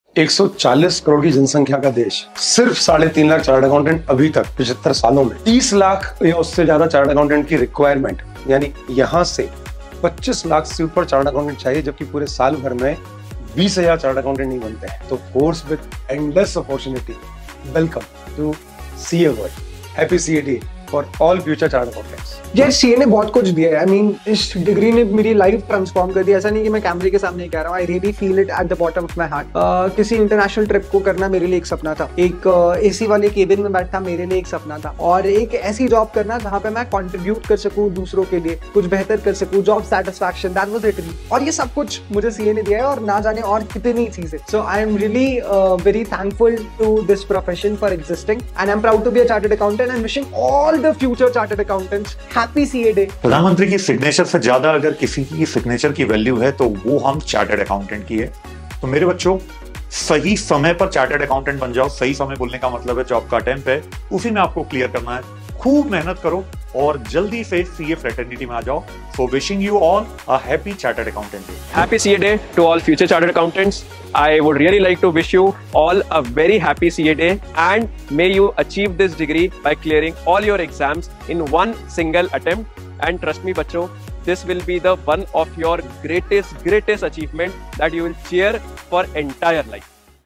Happy CA Day !
Wishing all the Chartered Accountants a Happy CA Day from Vidhyoday! Thank you for your hard work and dedication. Keep shining bright!
For Any Information related to:
Call timings: Monday to Saturday 8 AM to 8 PM